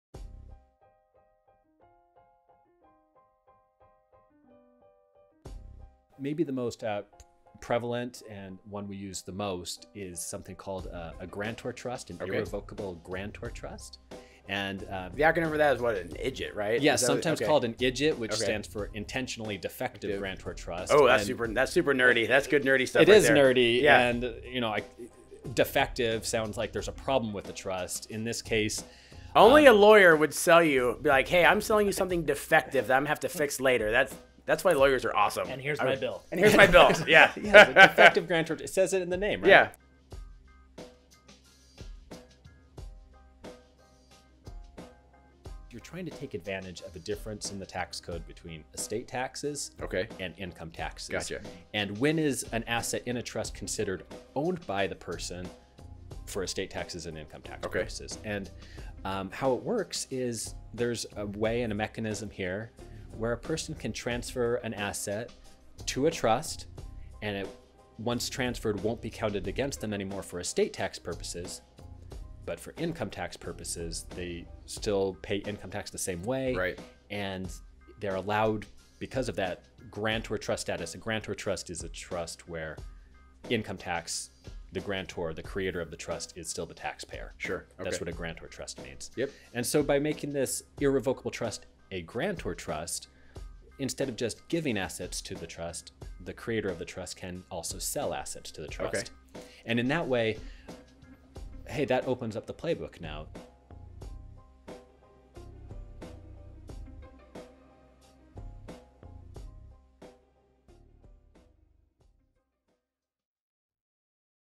Intentionally Defective Grantor Trust (IDGT) - Estate Planning Jargon
We sat down with Jeff Stephens and asked him the things to think about when looking for tax efficient planning. One of the trusts he brought up was the Intentionally Defective Grantor Trust. By having the option to sell assets versus give assets, the grantor can be more strategic. More details in the full episode, “Advanced Estate Planning, Charitable Strategies, and a Whole Lot of Acronyms!”